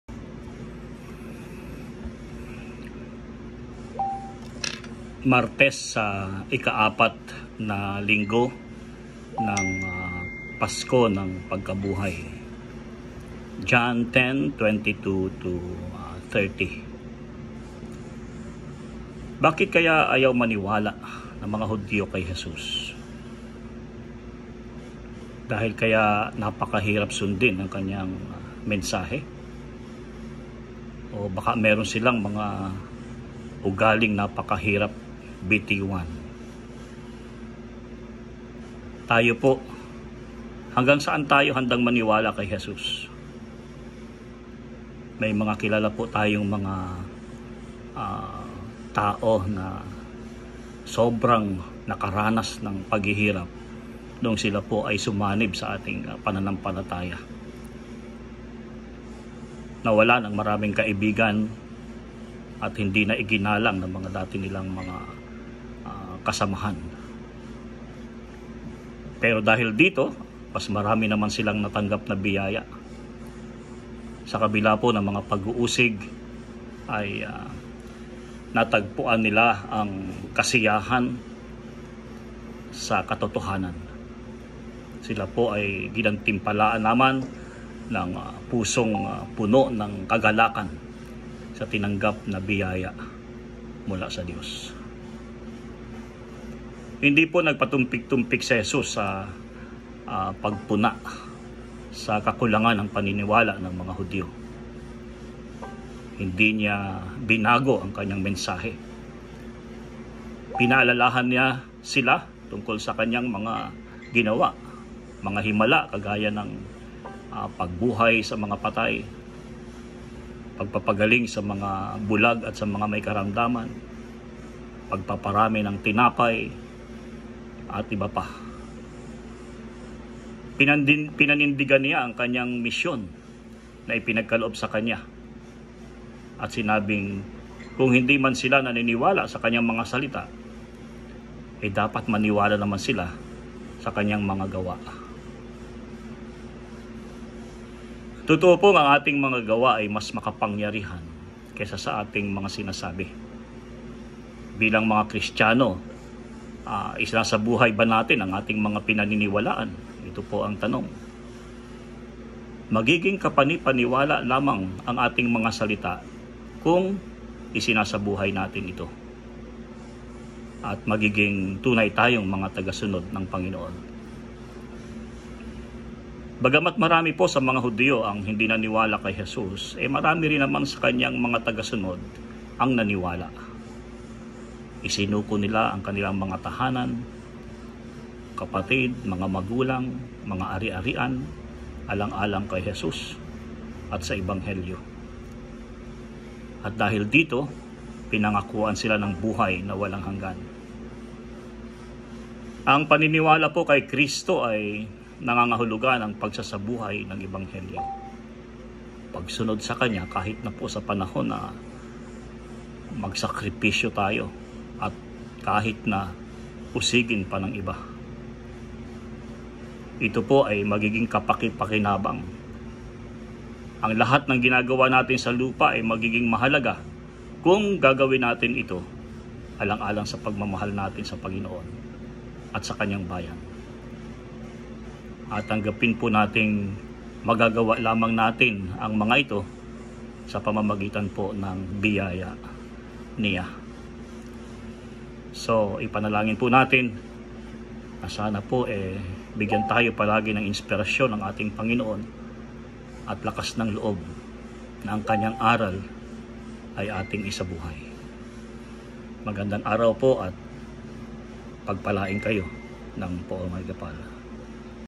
Martes sa Ikaapat na Linggo ng Muling Pagkabuhay. John 10:22-30
"Magiging kapani-paniwala lamang ang ating mga salita kung isinasabuhay natin ito. At magiging tunay tayong mga tagasunod ng Panginoon."